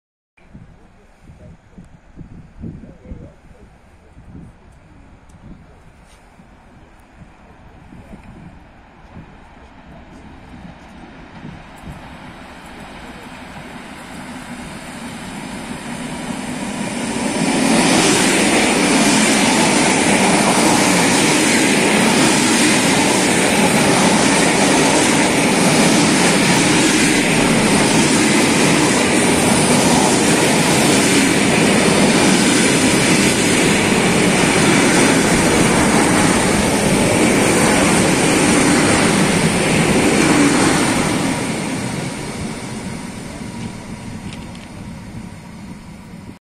Электропоезд ЭД4М-0395 сообщением Москва-Ярославская - Сергиев Посад
Электропоезд ЭД4М-0395 (ТЧ-10 Москва-II (МВПС)) сообщением Москва-Ярославская - Сергиев Посад прибывает на 2 путь ст. Софрино, п. Софрино, Пушкинский городской округ, Московская область, Ярославское направление МЖД. 14:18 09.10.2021 г.